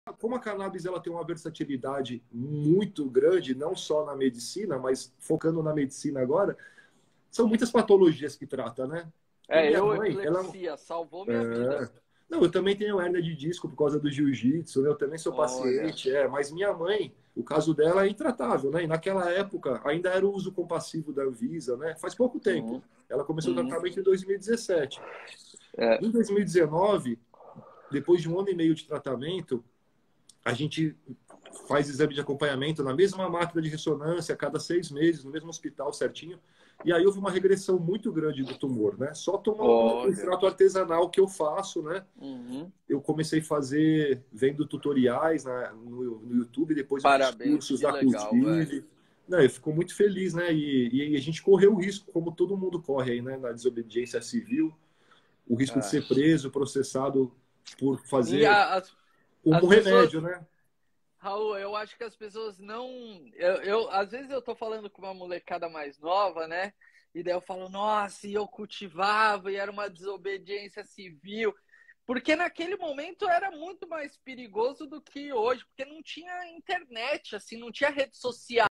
Raul Thame e João Paulo Ganja Talks LIVE corte 01
Saiba mais sobre a trajetória de Raul Thame e sua luta. A medicina e o esporte nacional com voz no congresso!